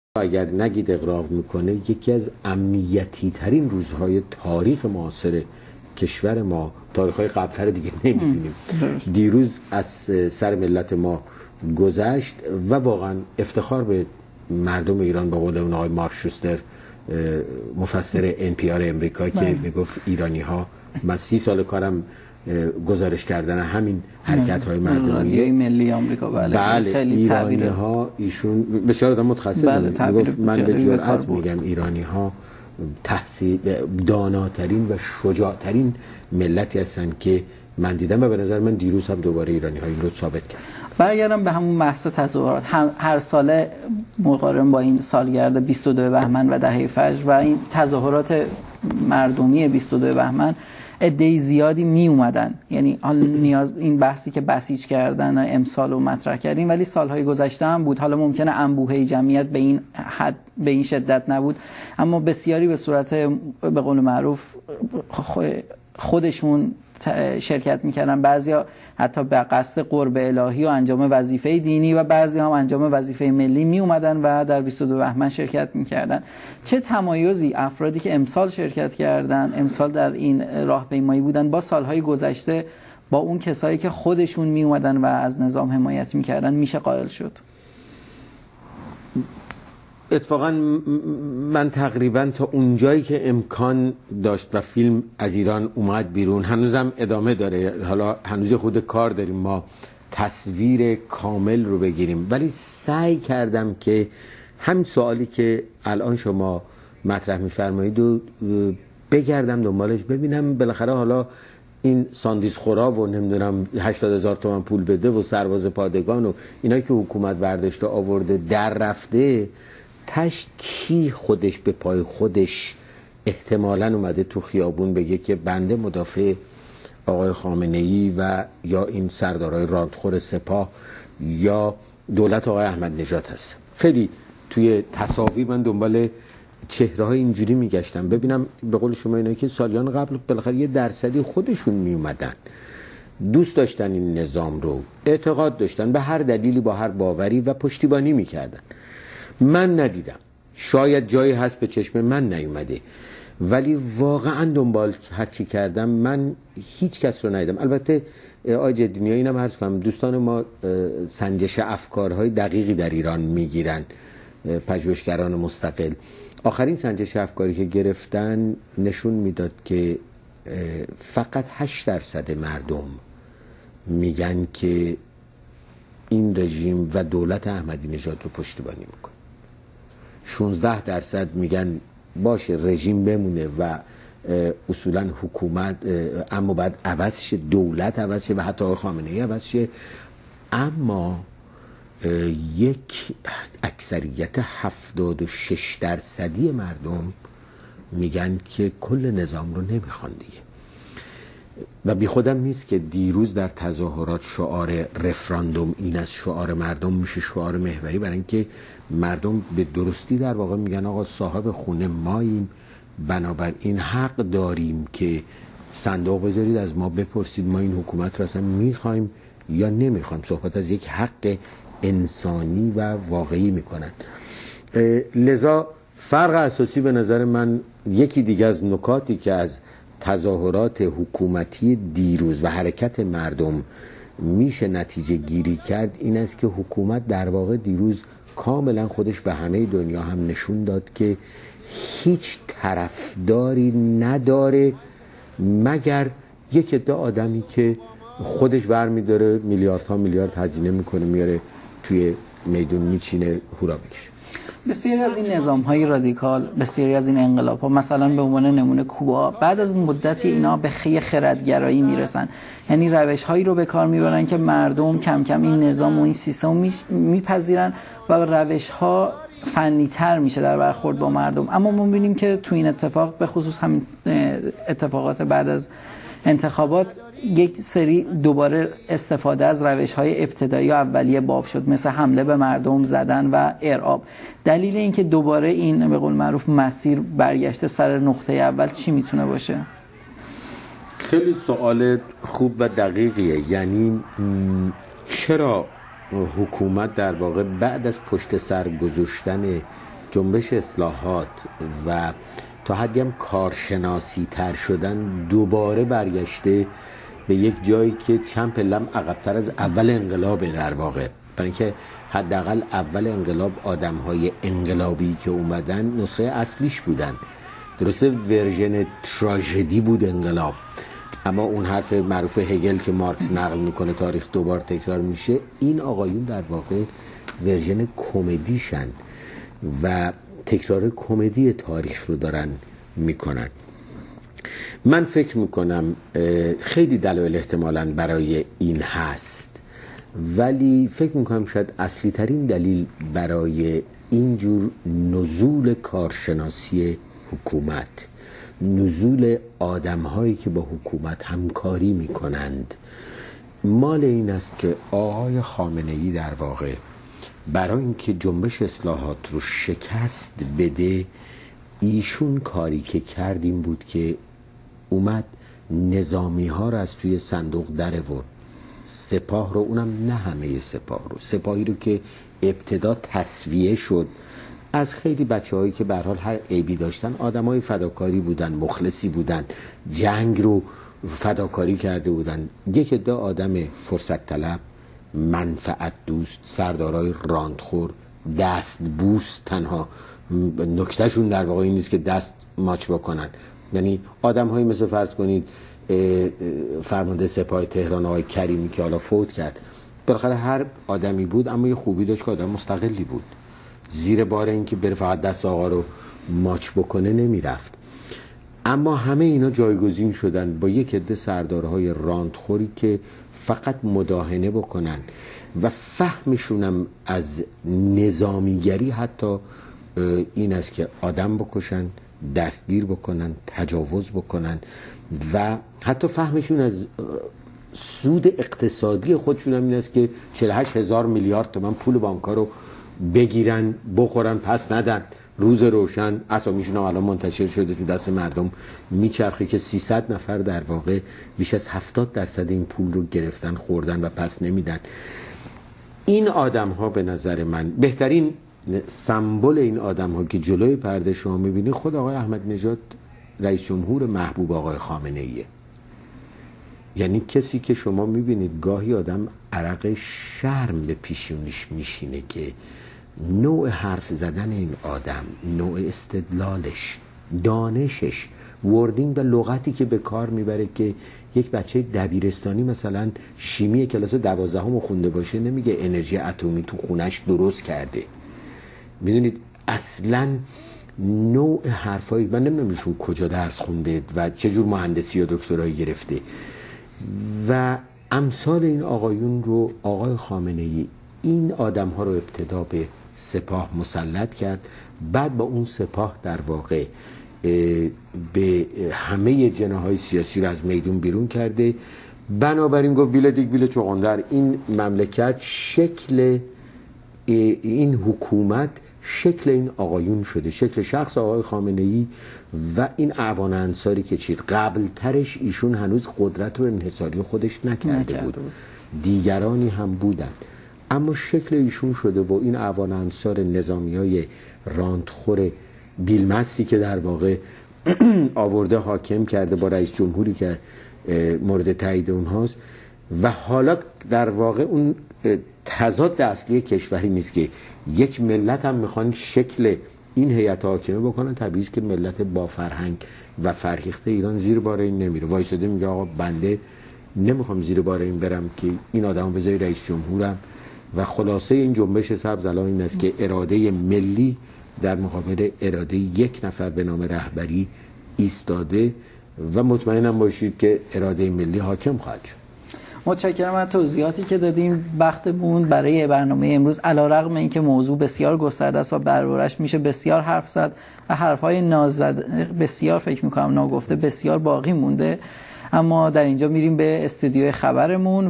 Sazegara-Jedinia 2.wmv
میز گرد جمعۀ وین تی وی
گفتگو با محسن سازگارا در بارۀ تظاهرات همآهنگ شدۀ 22 بهمن